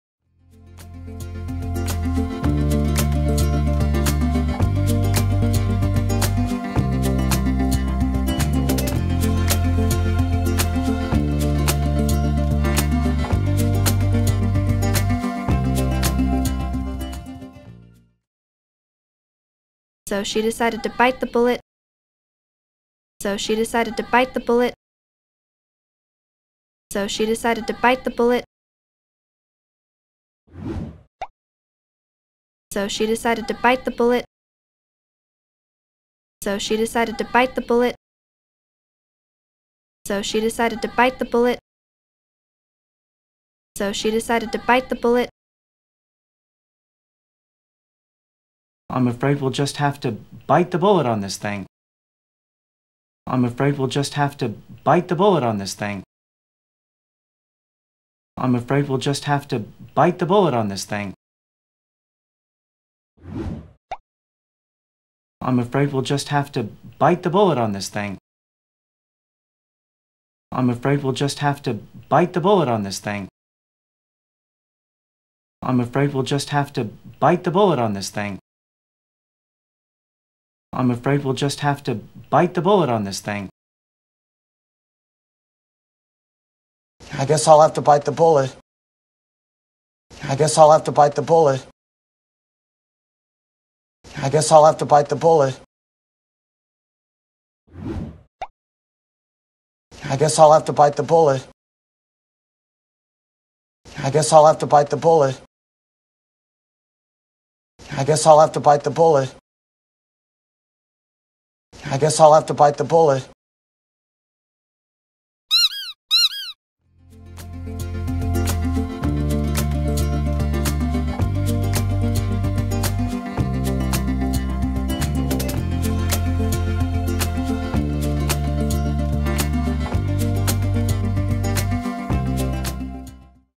[영어이디엄] bite the bullet 이를 악물고 참다
영어이디엄을 하루에 한마디씩 영화나 드라마를 통해 확인해 보면서 새로운 표현들을 꾸준히 배우고, 영어이디엄에 대한 감각을 터득해 나갈 수 있는 계기가 되었으면 합니다.